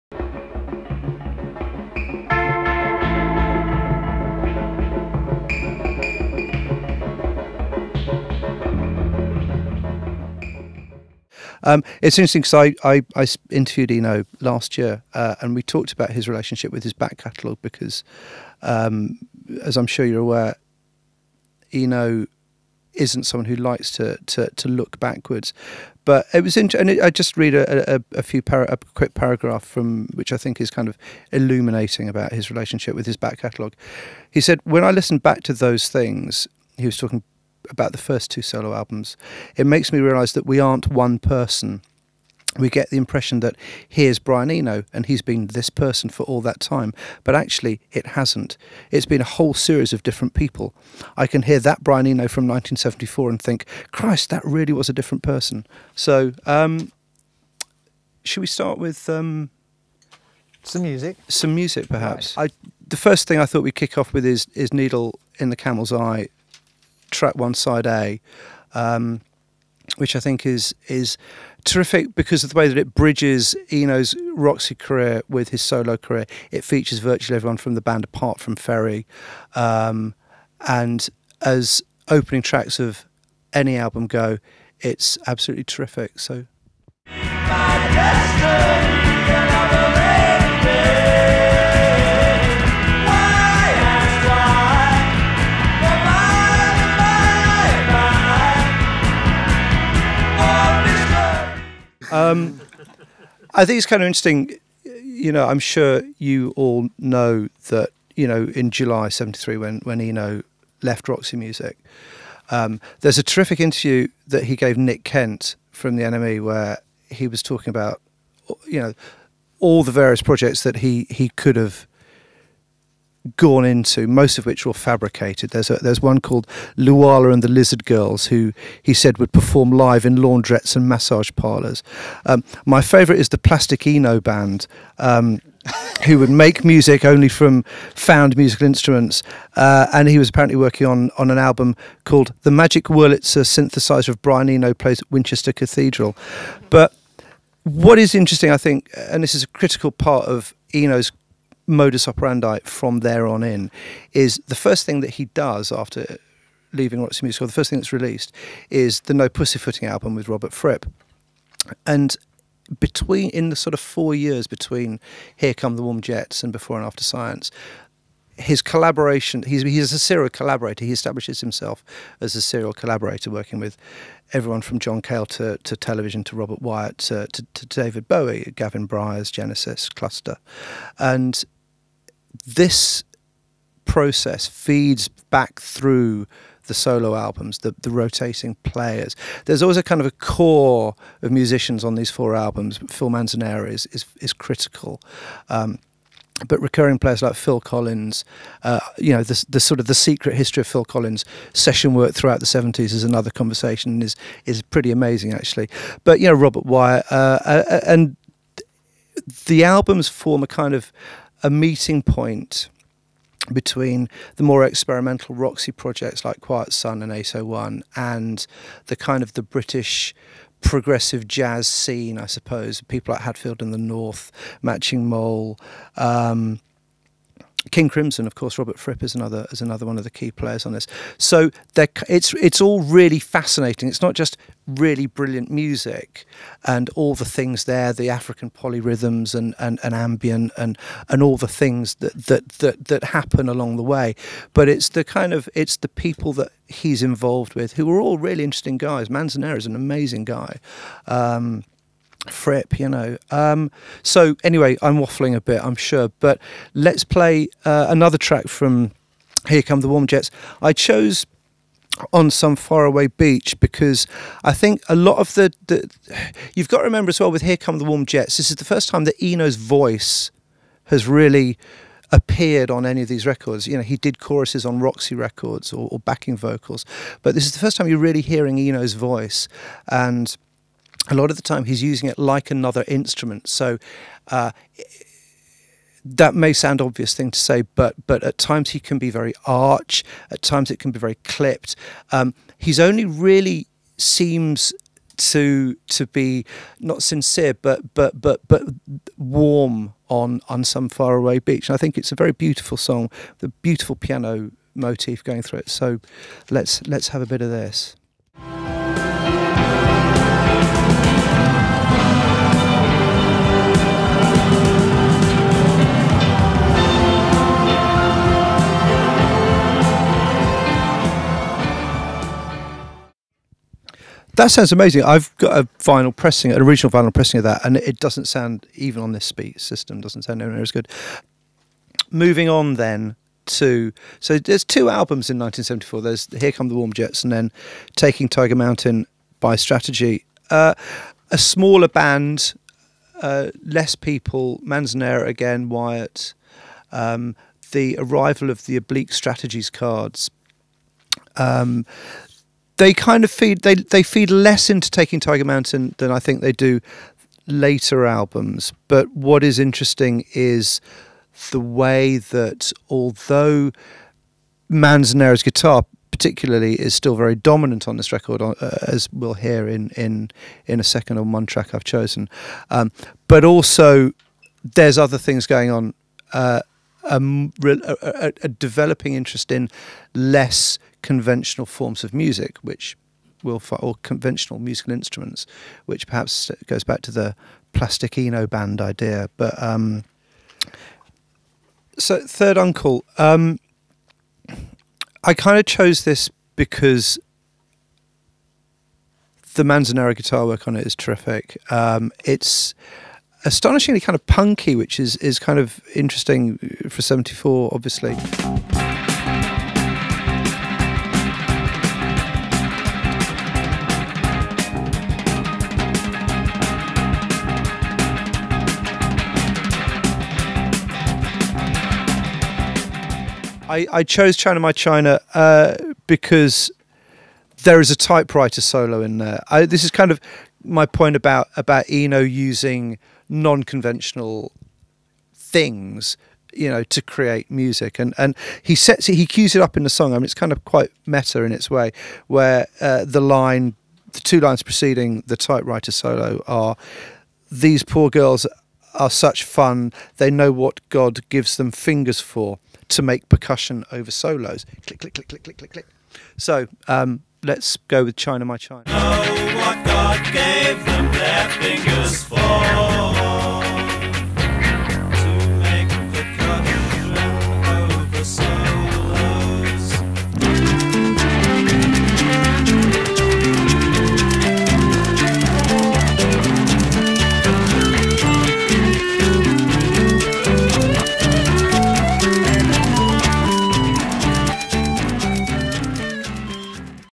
Brian Eno: Half Speed Mastering (Part 2)
In August, Uncut magazine’s Michael Bonner spoke to Miles Showell at Abbey Road Studios to celebrate the release of the Brian Eno Half-Speed vinyl remasters. The event was filmed for Brian’s fans so here’s Part 1 of the conversation, where Miles explains to Michael and a small, invited audience what half-speed mastering is. Parts 2 and 3 – coming soon – will dig into the importance of those timeless first four Eno albums.